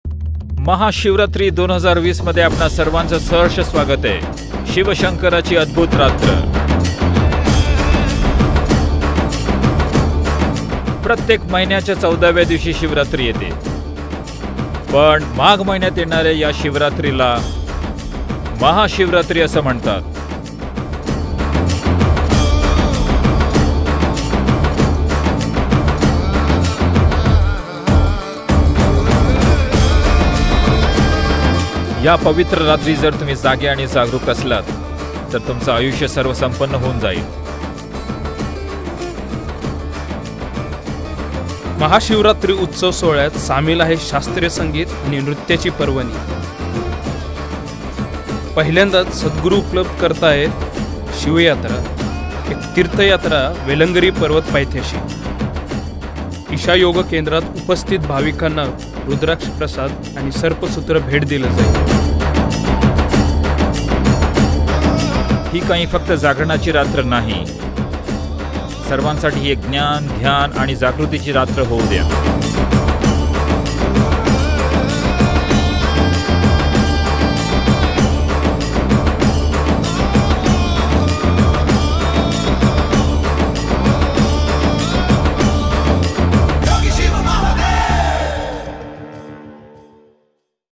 सद्गुरु आमंत्रित करताहेत आपल्याला महाशिवरात्री 2020 साठी Sadhguru Invites You To MahaShivRatri 2020!
साजरी करा महाशिवरात्रि २१ फेब्रुवारी २०२० रोजी ईशा योग केंद्रात. या उत्सव सोहळ्यात सहभागी व्हा प्रत्यक्ष ईशा केंद्रात किंवा लाईव्ह प्रसारण पाहायला विसरू नका प्रमुख टीव्ही चानल्स, युट्युब आणि फेसबुकवर.

English Video - Sadhguru Invites You To MahaShivRatri 2020!

Celebrate Mahashivratri on February 21, 2020 at Isha Yoga Center. Join the celebrations live at the venue or watch the live telecast on major TV channels, YouTube & Facebook.